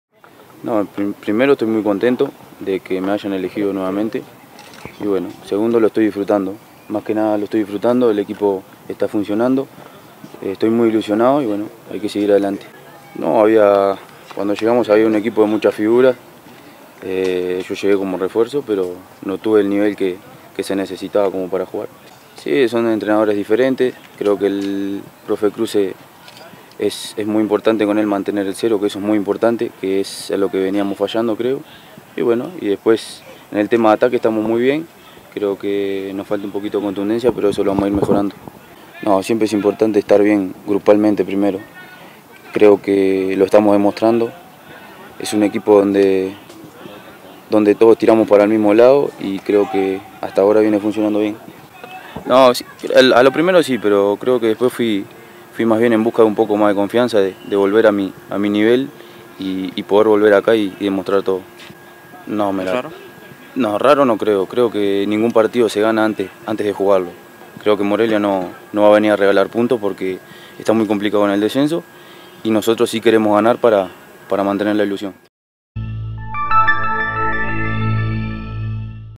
Christian Tabo está ilusionado con el desempeño del Atlas
APERTURA 2016. Guadalajara, México. 02/08/2016. El centrocampista uruguayo Christian Tabo, aseguró estar contento por el hecho de que el 'Profe' Cruz, lo haya tomado en cuenta para el cuadro titular  de los rojinegros y se dijo ilusionado con el desempeño del Atlas en el Apertura 2016.